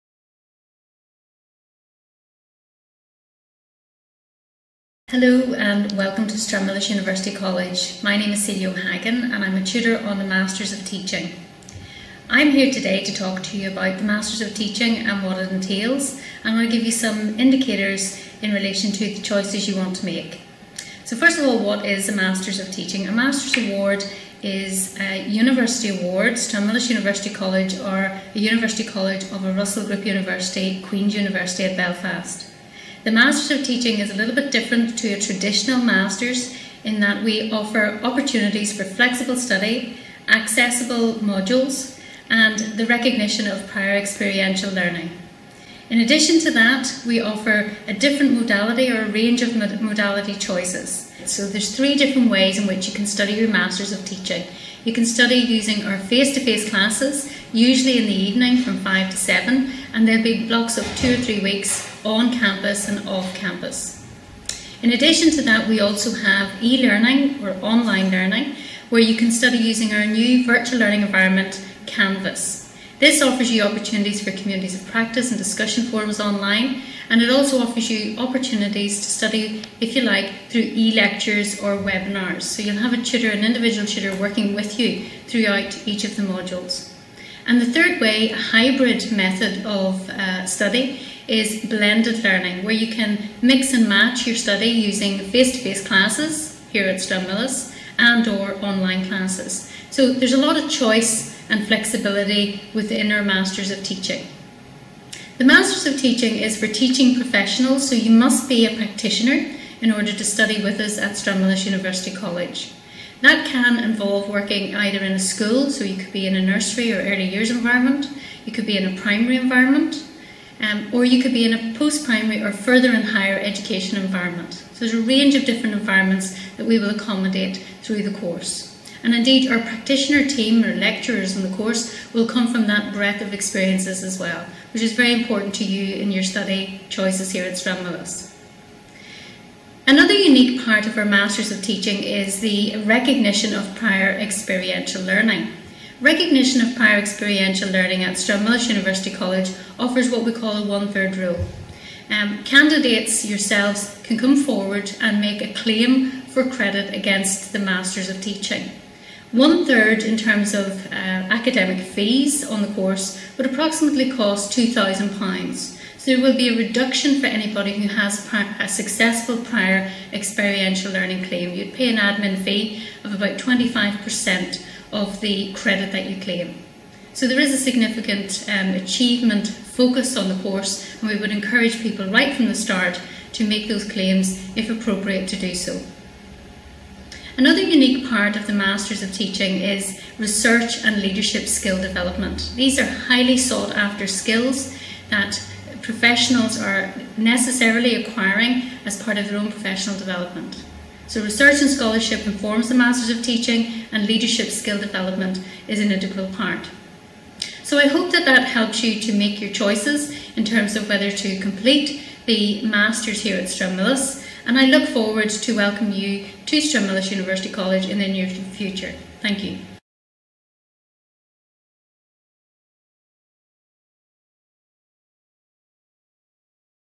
Find out more about our Master of Teaching (MTeach) programme
Our Master of Teaching (MTeach) programme offers a blend of learning opportunities for the developing teacher or leader in education who aims to advance their practices while working.

Studying at a distance means you can also visit Stranmillis for support and supervision, and you can utilise our online facilities.

If you prefer, you can study remotely by following an online programme of learning. MTeach students will have access to a single studies advisor throughout the programme, offering one point of contact and continuous learning support.